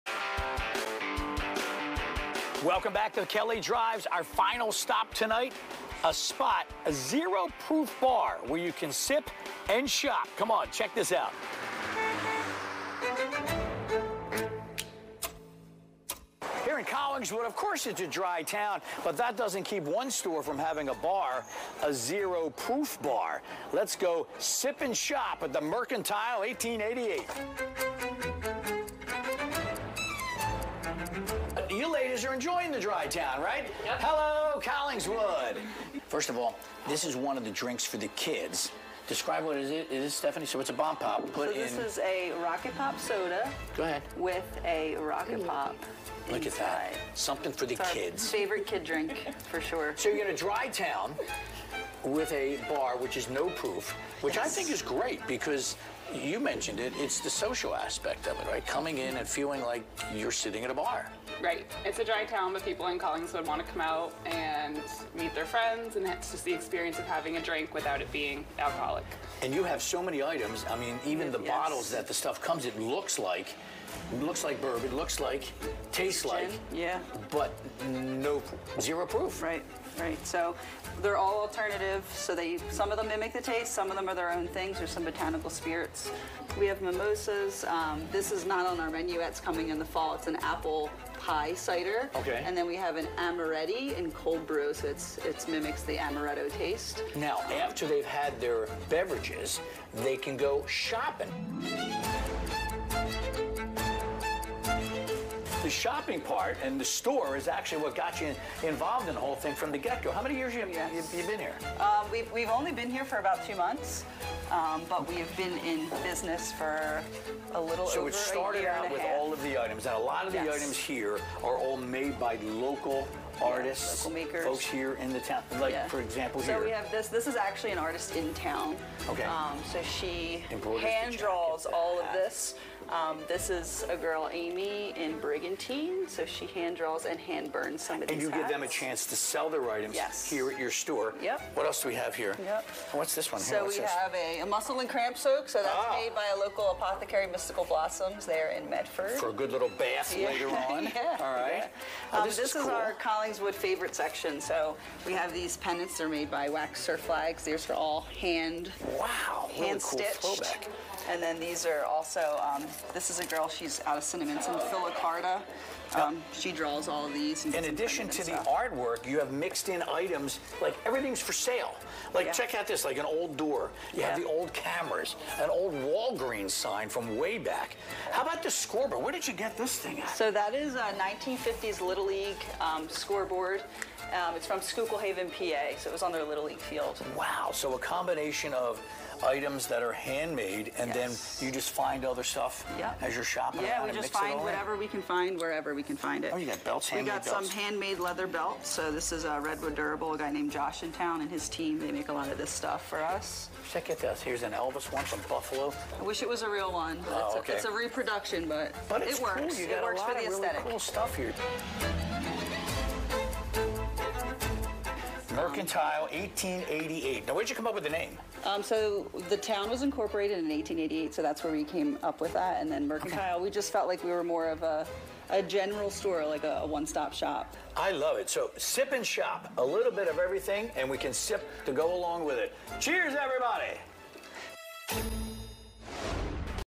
Zero Proof Bar at Mercantile 1888
Bob found a spot where you can sip on a non-alcoholic drink while you shop. He went to Mercantile 1888, which is not only a shopping destination but also a zero-proof bar serving non-alcoholic cocktails and beers.